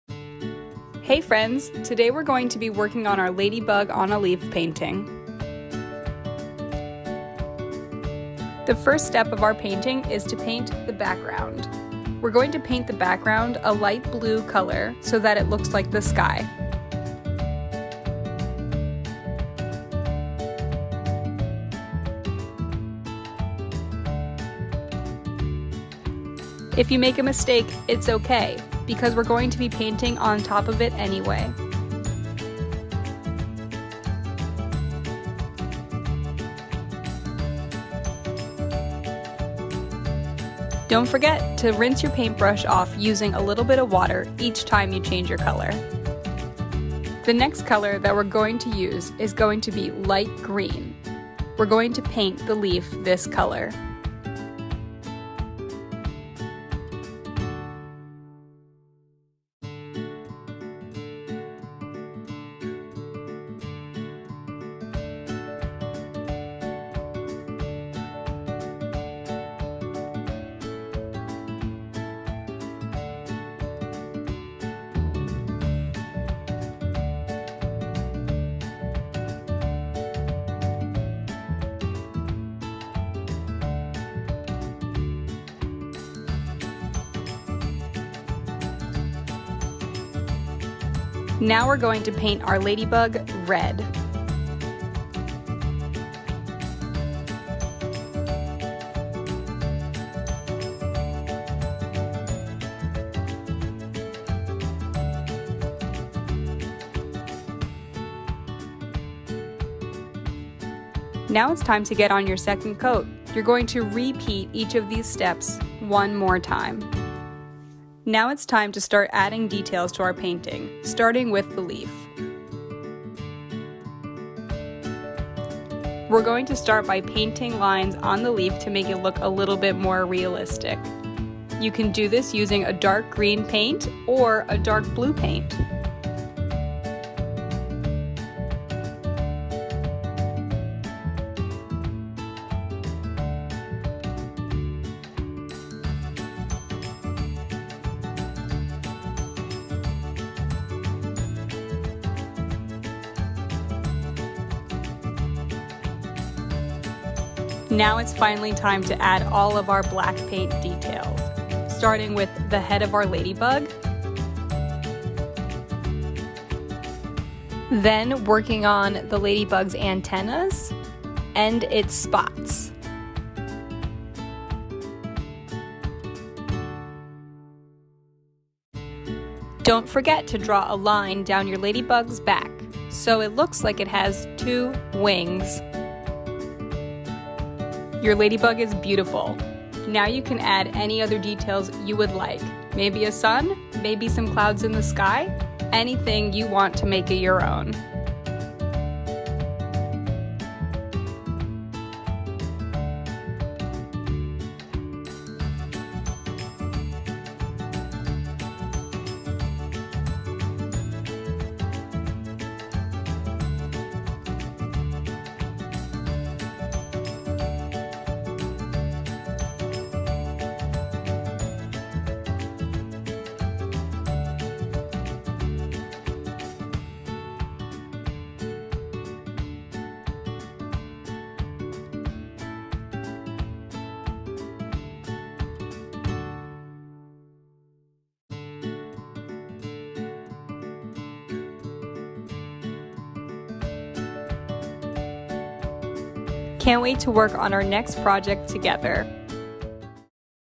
LADYBUG CANVAS PAINTING
Learn how to paint a ladybug painting on canvas